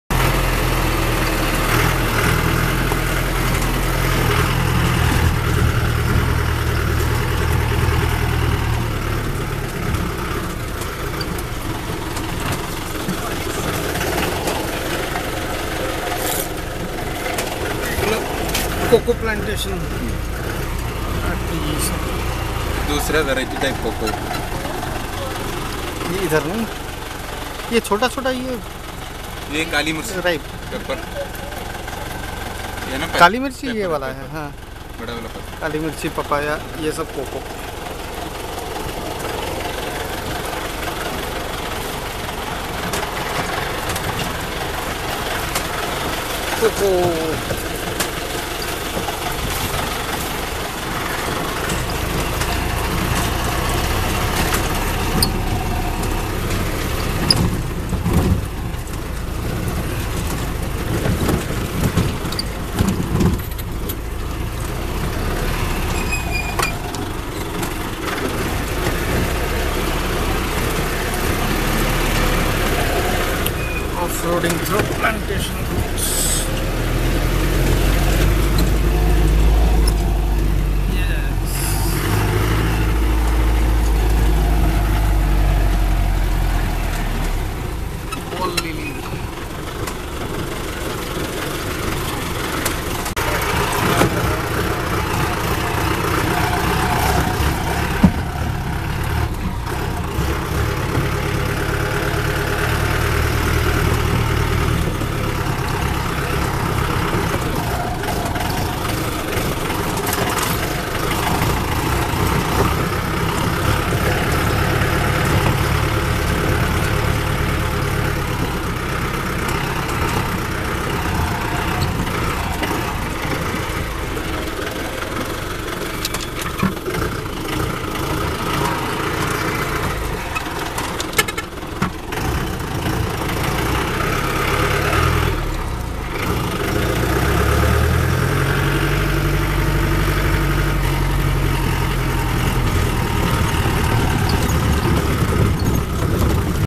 Going downhill through plantations of Munnar
Going downhill through plantations of Munnar in a 4x4 drive Jeep